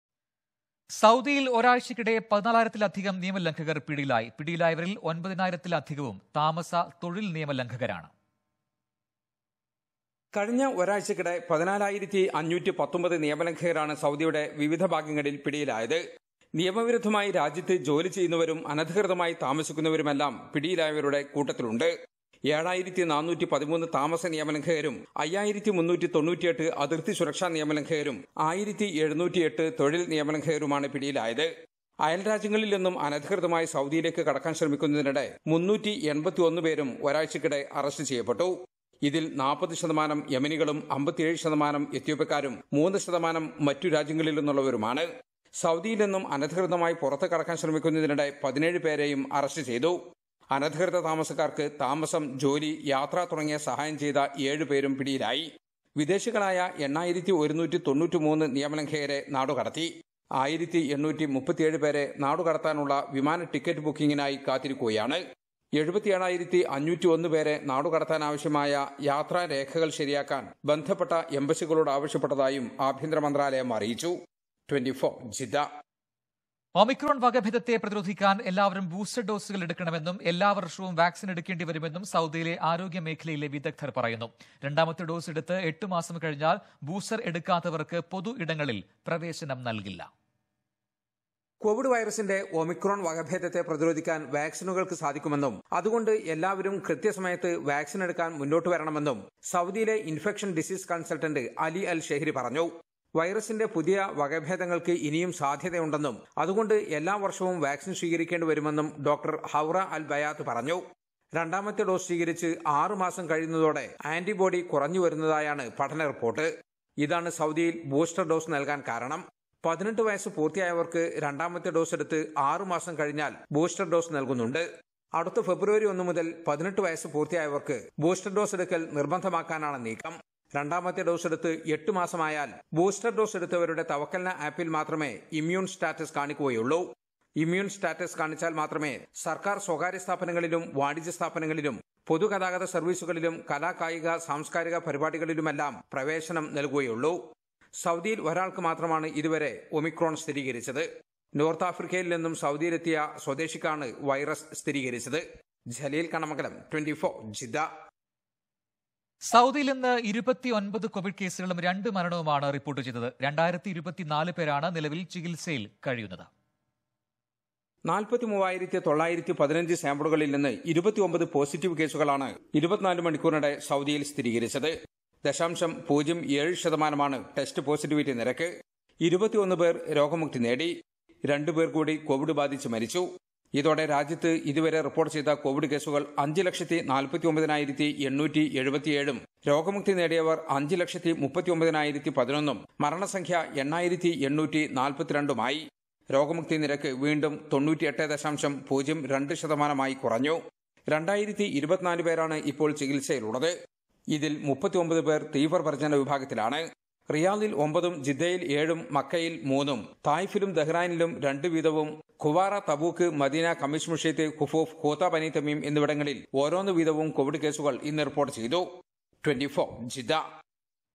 നിയമലംഘകരെ കണ്ടെത്താന്‍ പരിശോധന ശക്തം. illegals arrested
സൌദിയില്‍ നിയമലംഘകരെ കണ്ടെത്താന്‍ പരിശോധന തുടരുന്നു. ആയിരക്കണക്കിന് തൊഴില്‍ നിയമലംഘകരും താമസ നിയമലംഘകരും ഒരാഴ്ചക്കിടെ പിടിയില്‍. എണ്ണായിരത്തിലേറെ പേരെ നാടുകടത്തി.